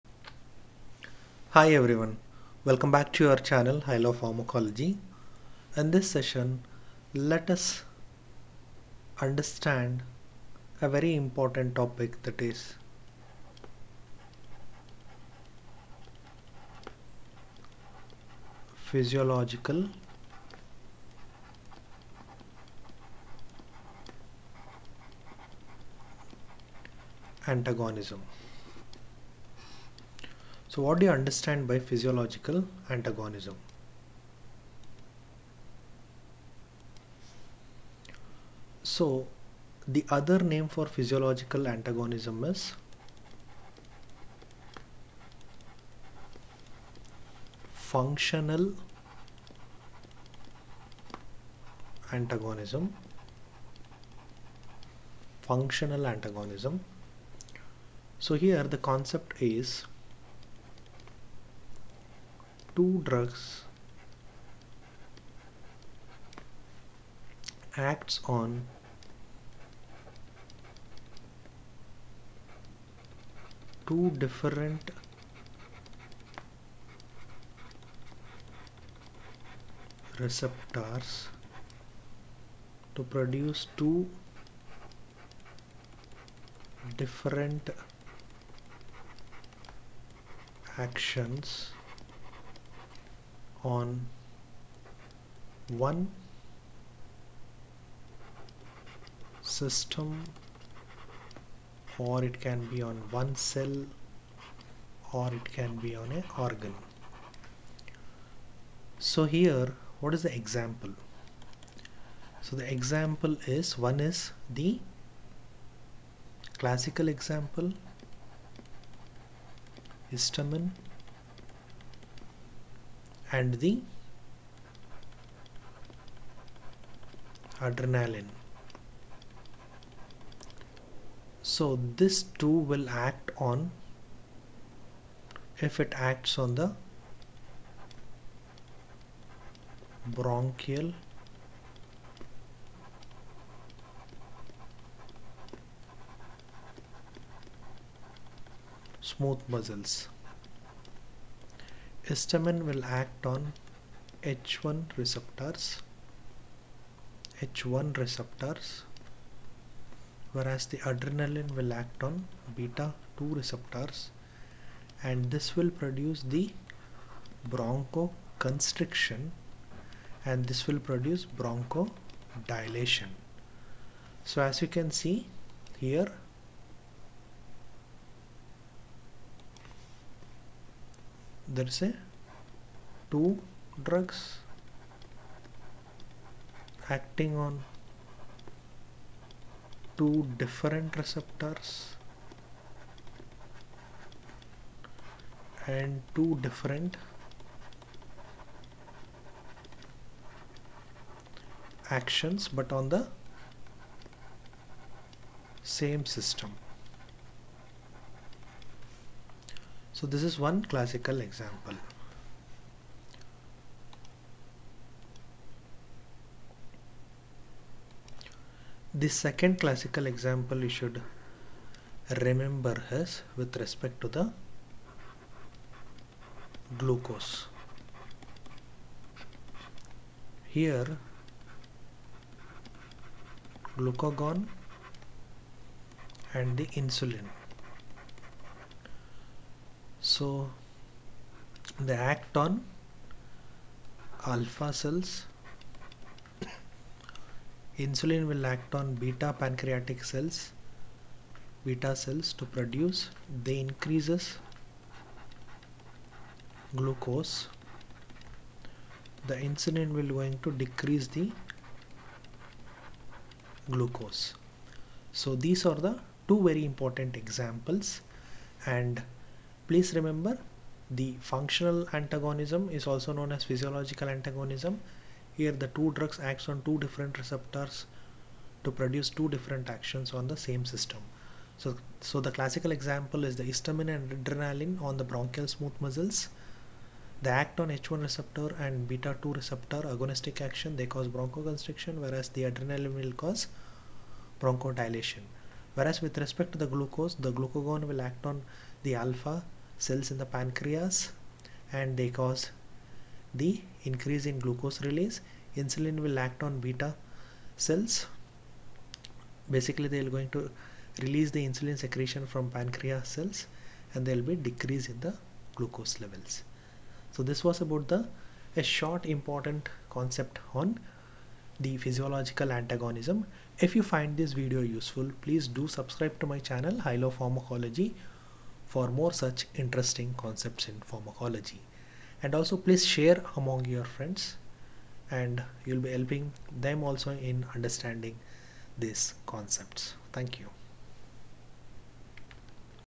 Physiological Antagonism (Made Easy) - Concept Explained (Functional Antagonism)
This video is about Physiological Antagonism - Concept Explained (Functional Antagonism) with examples